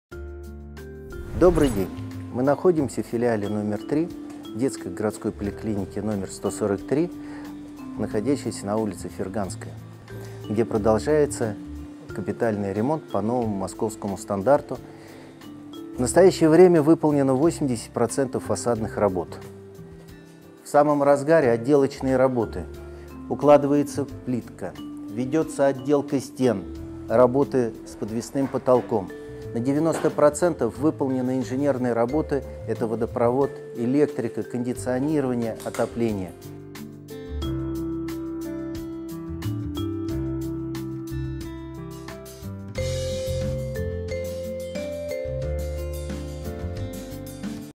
Капитальный ремонт филиала 3 ДГП №143
🛠 Мы продолжаем следить за ходом капитальных работ в ДГП 143 филиале №3 по новому московскому стандарту поликлиник.
🏥Выполнено 80% фасадных работ.
🚧Продолжается внутренняя отделка помещений: на пол укладывается плитка, ведутся работы с подвесным потолком и отделка стен.
🔌На 90% выполнены инженерные работы: водопровод, электрика, кондиционирование и отопление.
Следите за ходом капитального ремонта вместе с нами😊❗